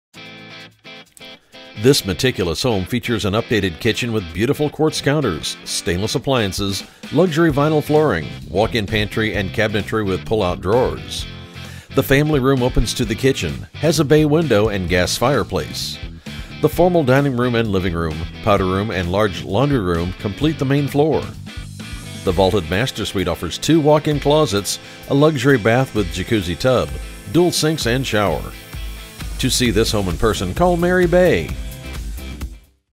16349 Black Cherry Drive Wildwood, MO | CBGundaker.com
16349 Black Cherry Drive, Wildwood, MO

Meticulous home features updated kitchen with beautiful quartz counters, stainless appliances, luxury vinyl flooring, walk-in pantry, and cabinetry with pull out drawers. Main floor has hardwood flooring in family room, living room and dining room, plus updated lighting throughout! The family room,